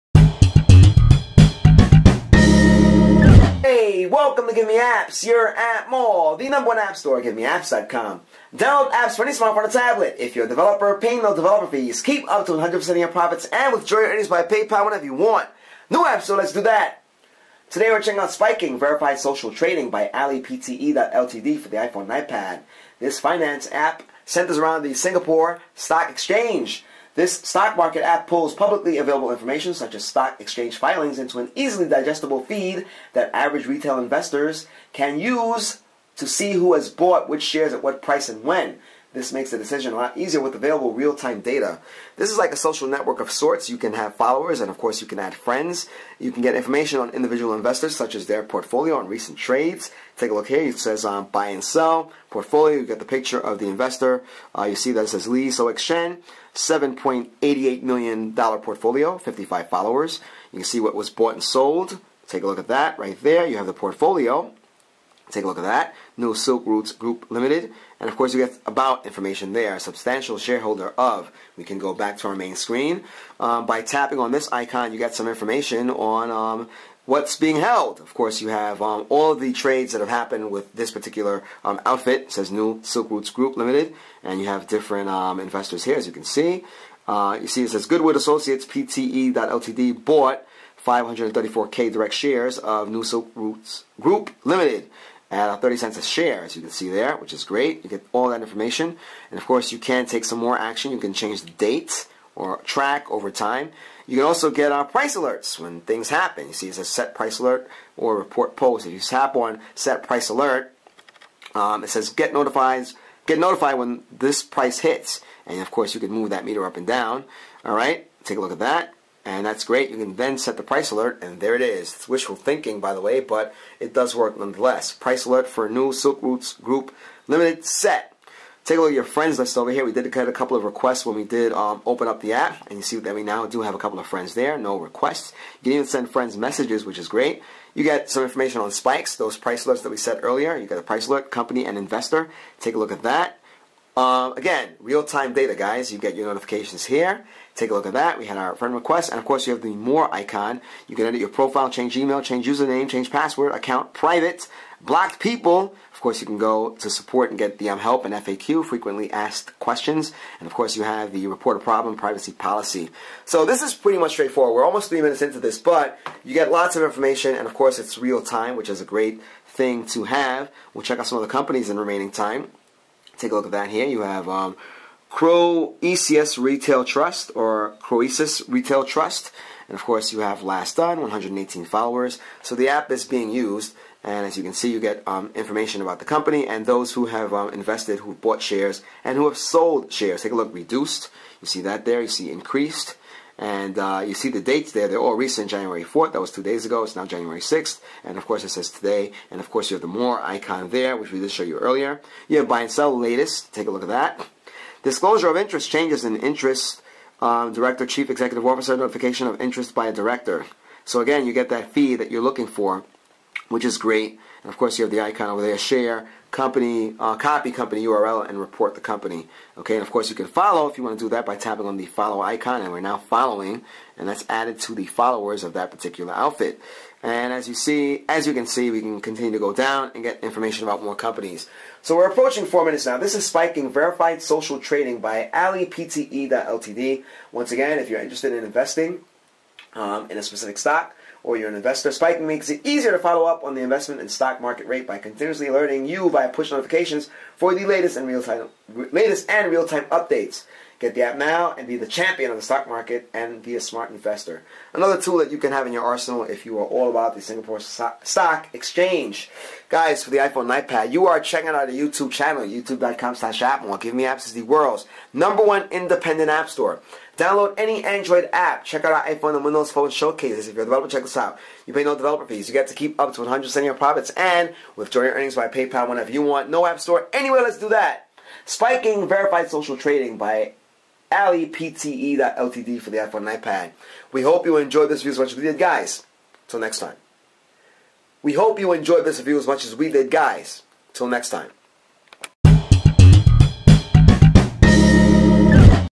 Spiking - Verified Social Trading iPhone/iPad App Review | GiveMeApps
Spiking - Verified Social Trading Android App Review by GiveMeApps

Spiking is a social trading app which pulls publicly available information such as stock exchange filings, trades and more and organizes it into an easy to follow newsfeed! If you've used any social network, you'll find getting around it super familiar!

This social trading app lets you not just see who purchased what and when, but it also allows you to message, follow and friend investors and companies! Even set alerts when shares are sold at a set price! Understanding the stock market has never been this easy!

With Spiking, you can follow the Singapore stock exchange with ease and network your way to stock market success!

At GiveMeApps you can sell iOS, Android and Windows Phone apps and keep 100% of your earnings.

Go ahead and get your mobile apps professionally reviewed by GiveMeApps team